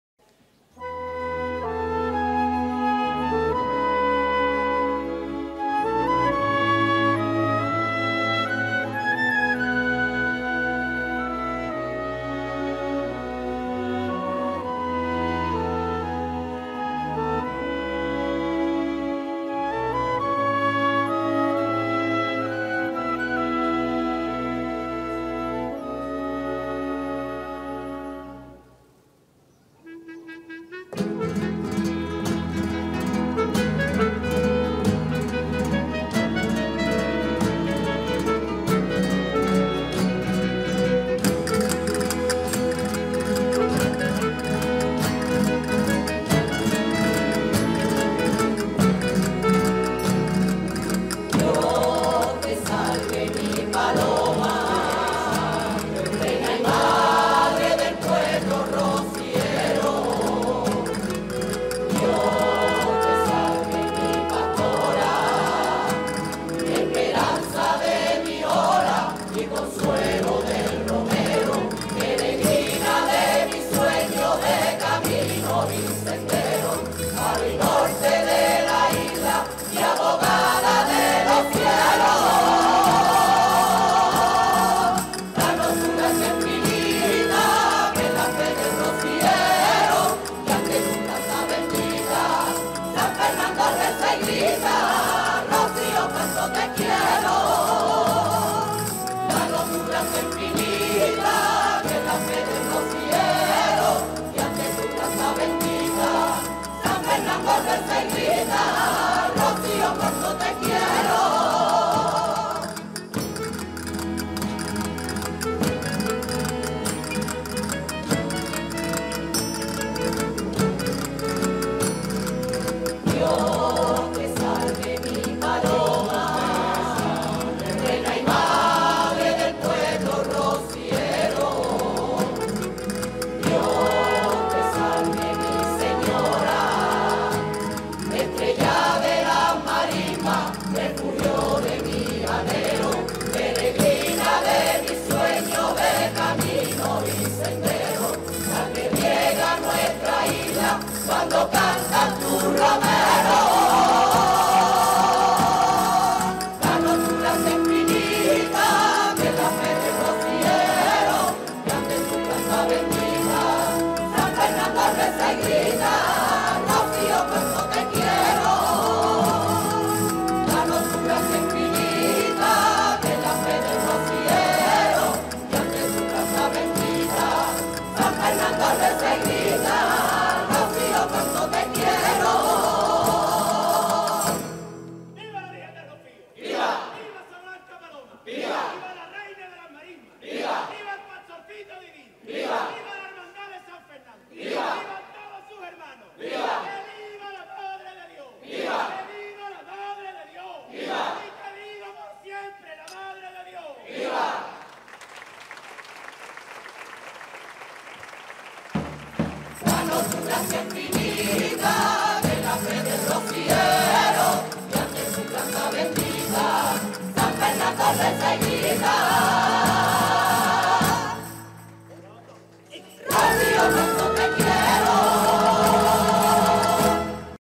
Saludo de la Virgen del Rocío ante el Simpecado de la Hermandad del Rocío de San Fernando | 2024
Saludo de la Virgen del Rocío ante el Simpecado de la Hermandad del Rocío de San Fernando en la procesión con motivo de Pentecostés.